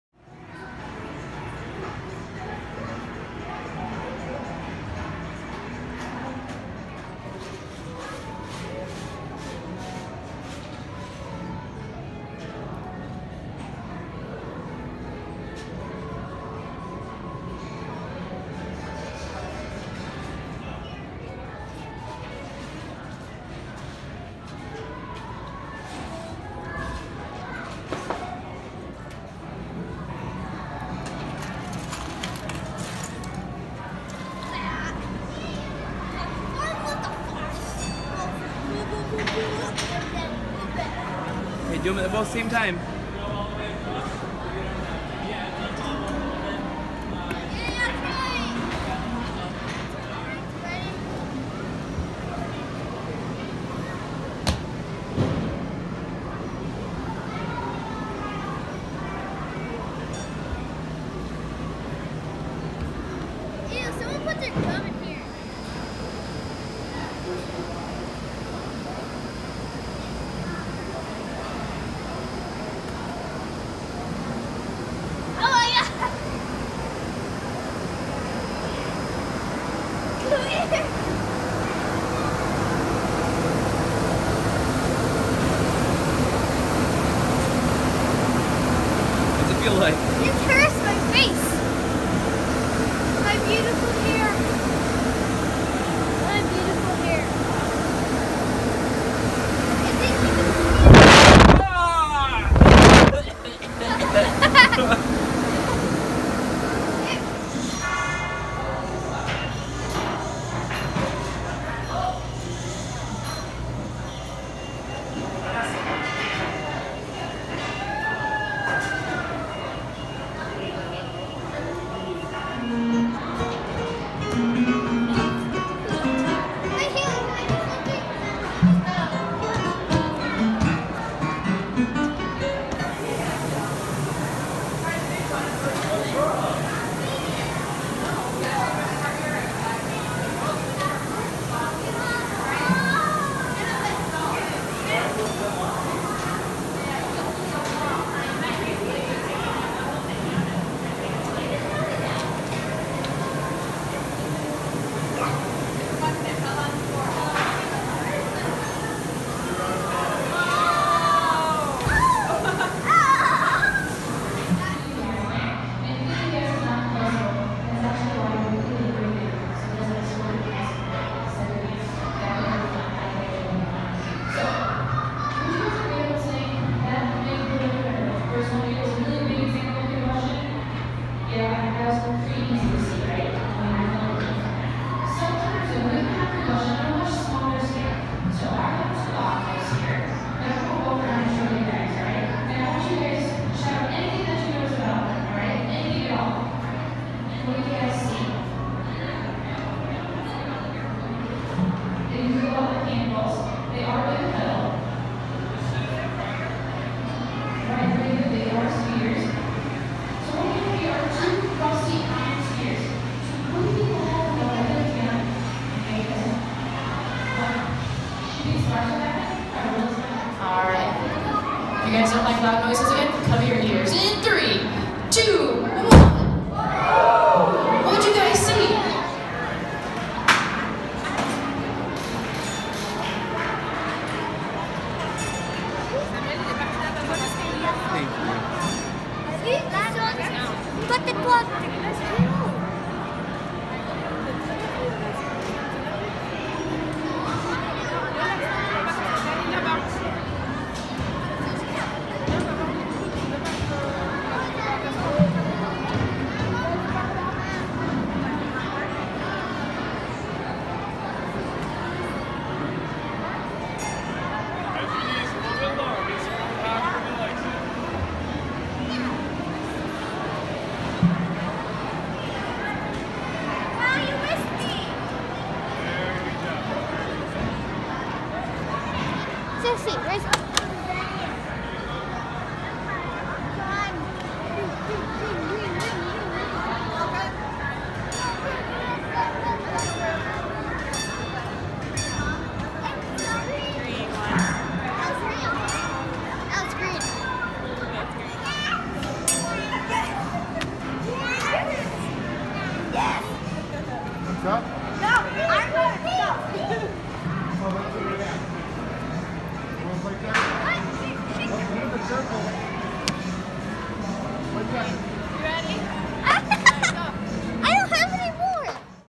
BB40 | Day 191 — Imagination Station
Went to a children's science museum.